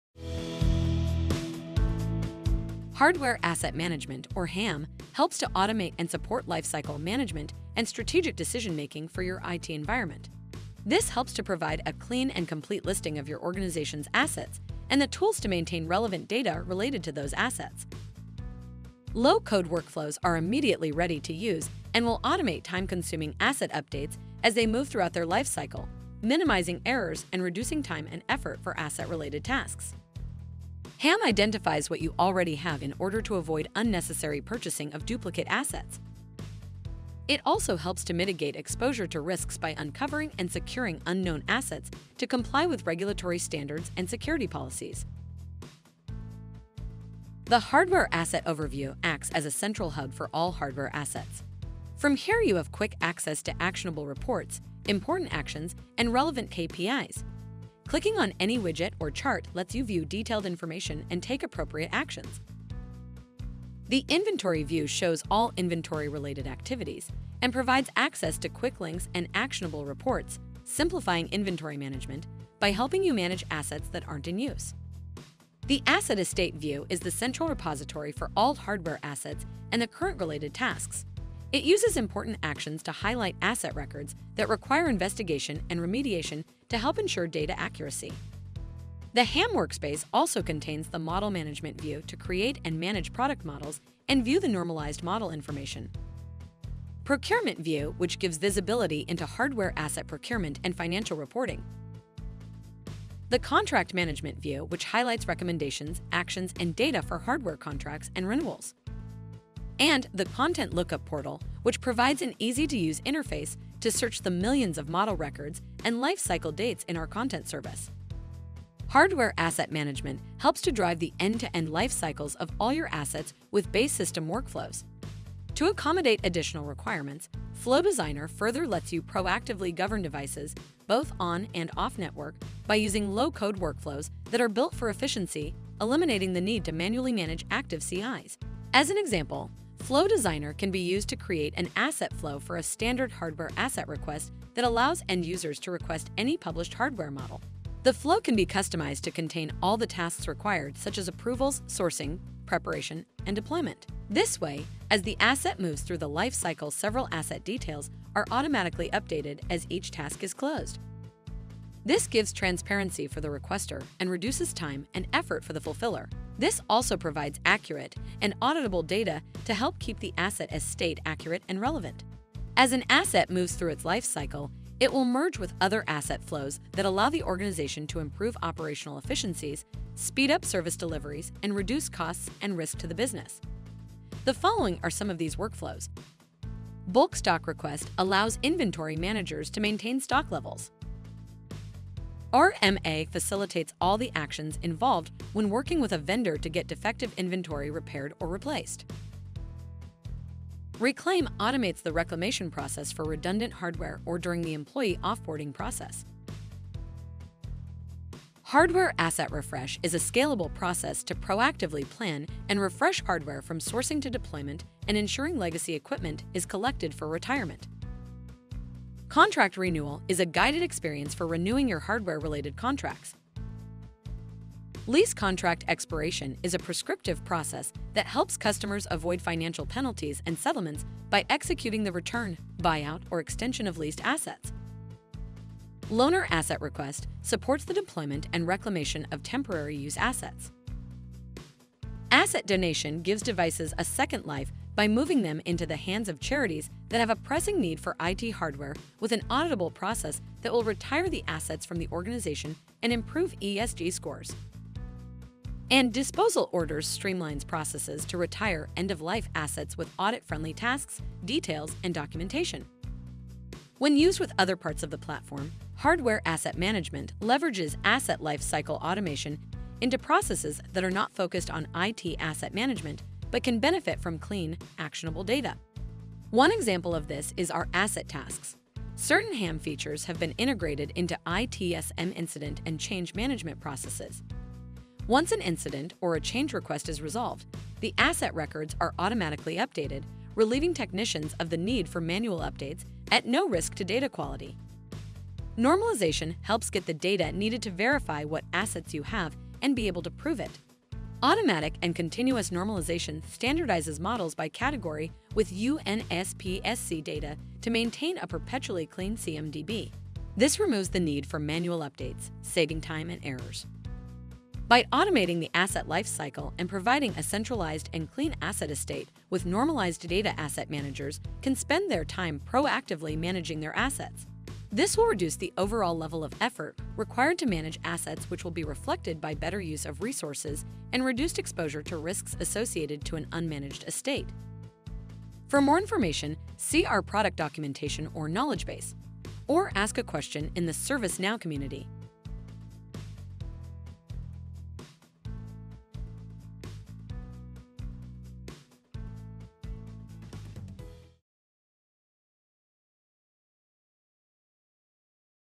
Hardware Asset Management | Overview
Hardware Asset Management, or HAM, helps to automate and support lifecycle management and strategic decision-making for your IT environment.

This video covers:
00:00 Intro
00:46 Hardware Asset Workspace
01:03 Inventory view
01:15 Asset estate view
01:28 Model management view
01:36 Procurement view
01:43 Contract management view
01:49 Content lookup view
01:58 End-to-end lifecycles
02:52 Asset workflows
04:55 Normalization